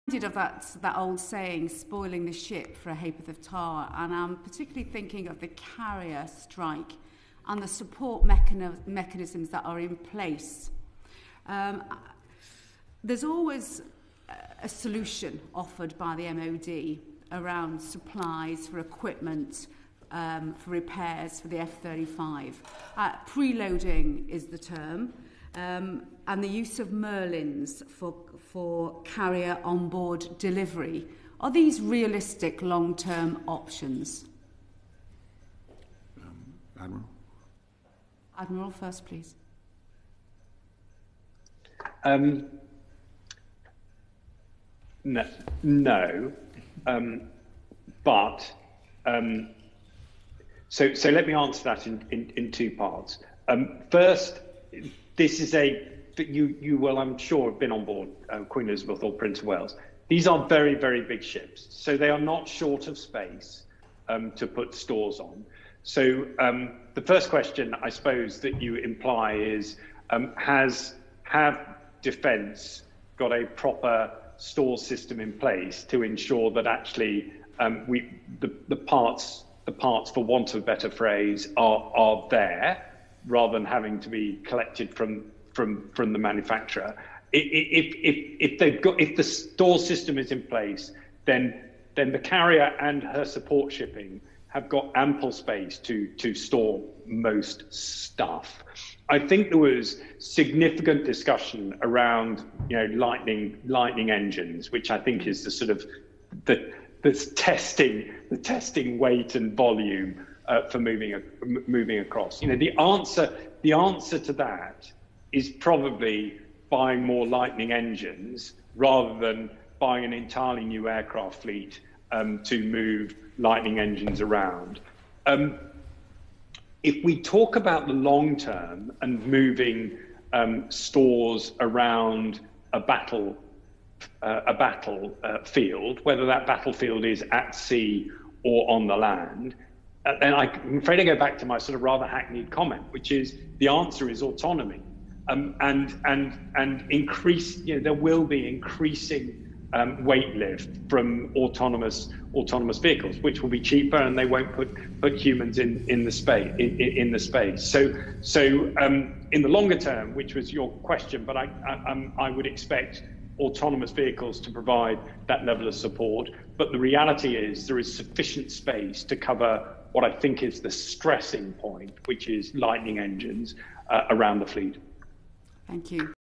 'Defending Global Britain in a Competitive Age', Question Two, 13th April 2021
Sarah Atherton Question One to the Defence Select Committee's inquiry on 'Defending Global Britain in a Competitive Age', 13th April 2021